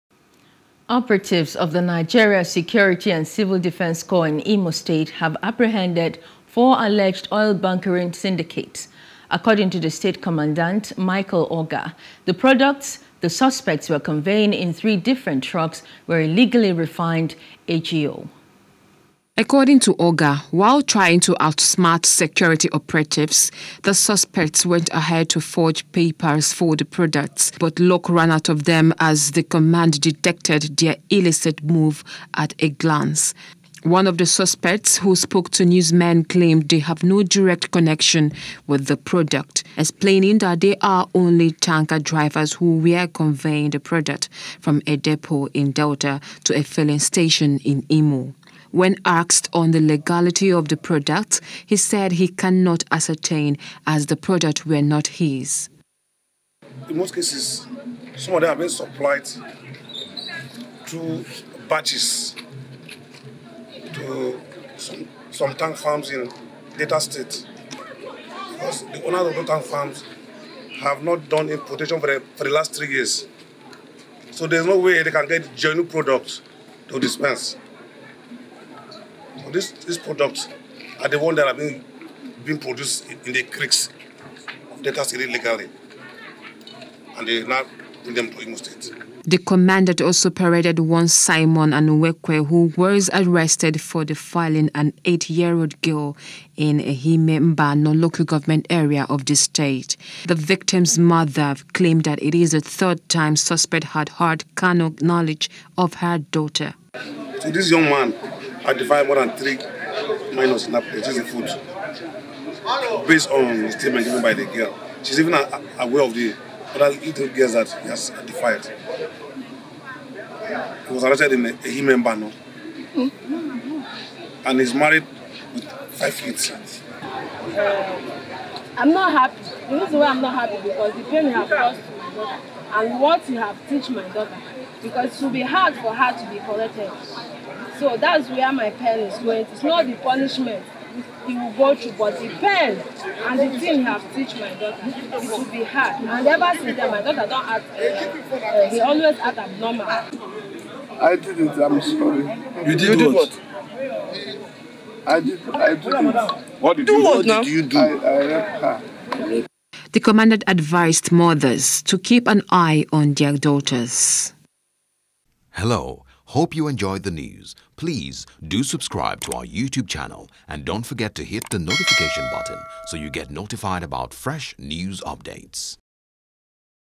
N.S.C.D.C Apprehends Suspects For Illegal Oil Ideas, Defilement | NEWS
Operatives of the Nigeria civil defense corps Imo state command under the leadership of commandant Micheal Ogar have apprehended four alleged notorious oil bunkery syndicates in the state.